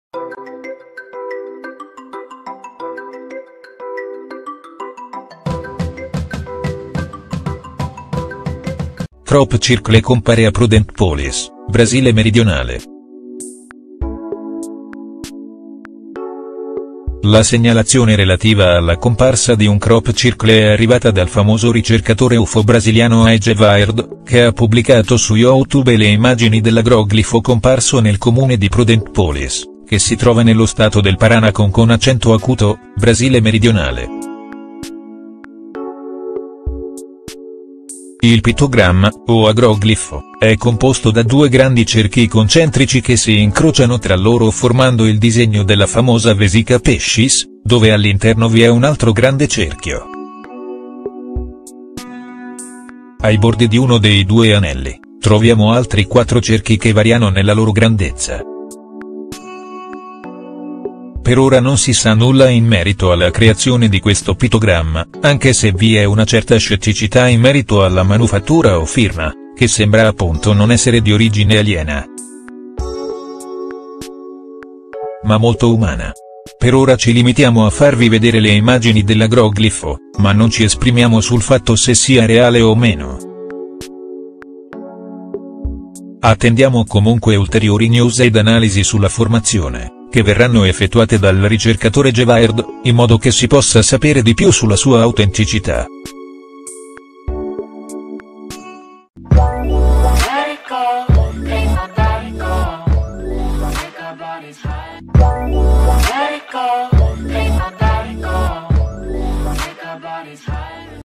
Crop Circle compare a Prudentópolis, Brasile meridionale
La segnalazione relativa alla comparsa di un crop circle è arrivata dal famoso ricercatore UFO…